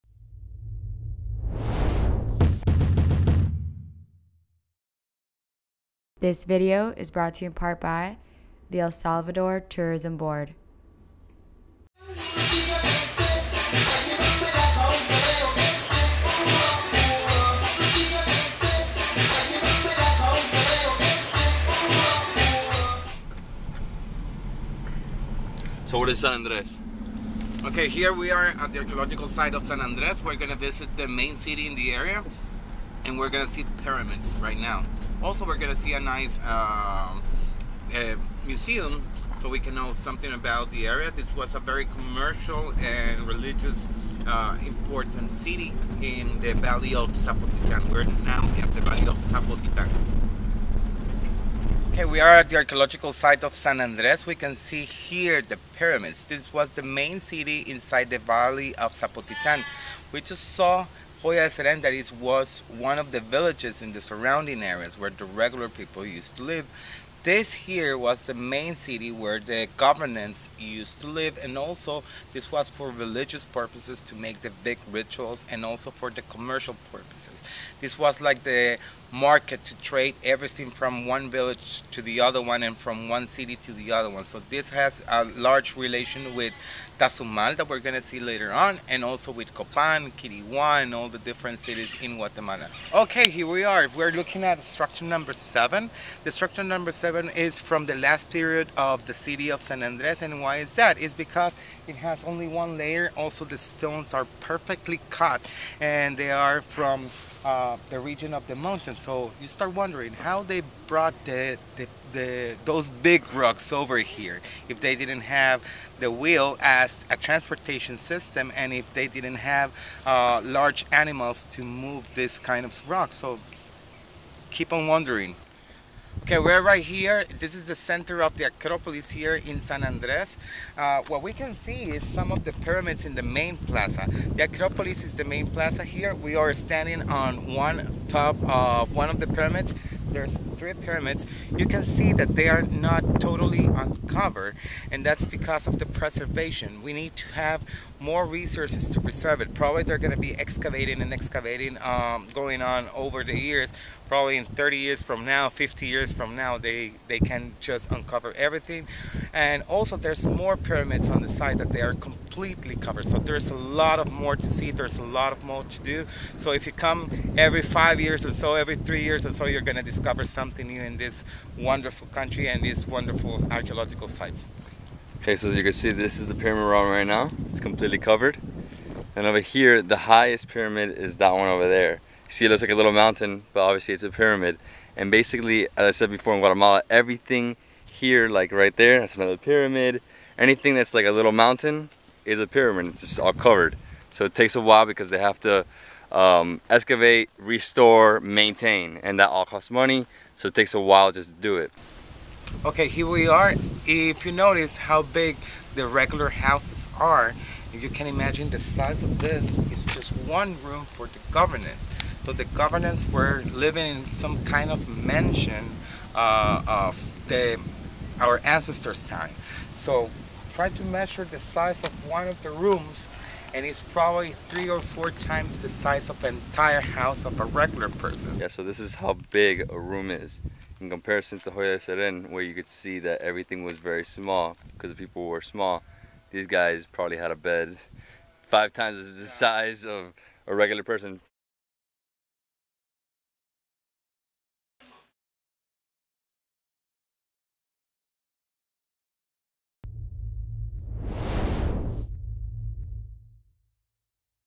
San Andres "Mayan Ruins" El Salvador
DBH Unplugged continues its archaeological journey through El Salvador to the ancient city of San Andres. Dating back to 900 BC, this city was once an important commercial and religious center for the Mayan people, housing the government body and controlling trade between surrounding villages and cities. Tour through the site as David displays the ruins of government houses, the main plaza and pyramids dotting the site. With most of the city still undergoing excavation, there's something new to experience every time you visit. Tour this amazing archaeological site and learn even more about the ancient Mayan world with the DBH Guide: El Salvador, also available for your Kindle.

SEND ME STUFF:
USA